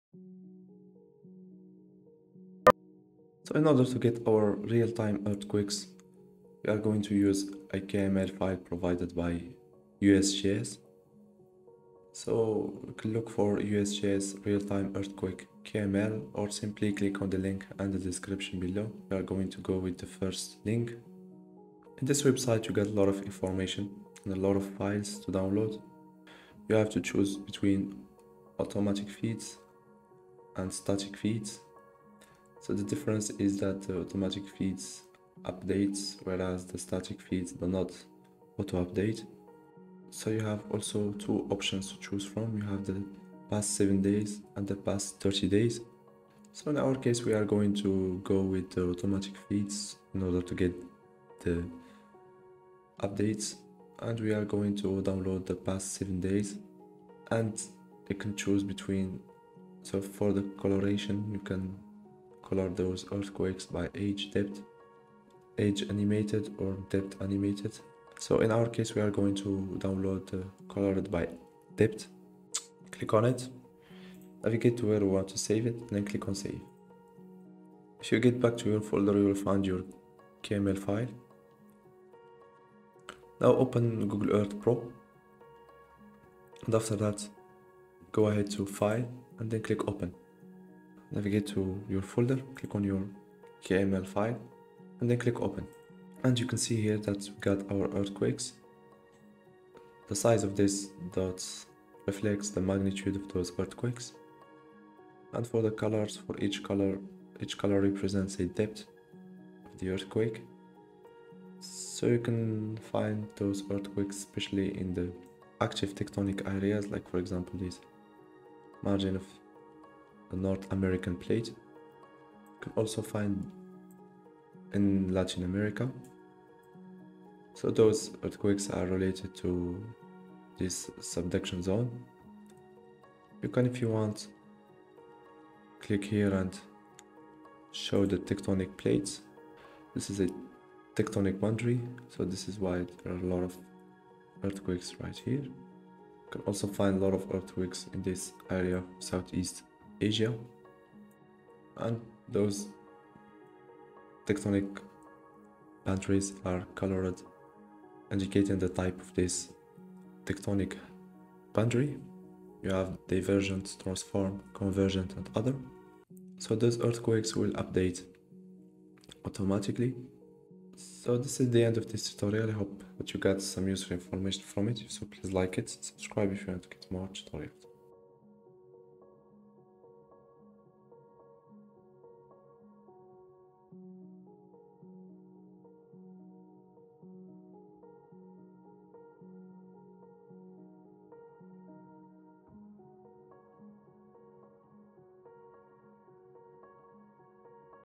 Get Latest Earthquakes as KML file
UGSG platform contains various datasets, in this tutorial we are going to use this website to download free Google Earth KML file. The KML feeds offer a variety of options, you can view earthquakes colored by age or depth, and an animated feed allows you to animate the series of earthquakes.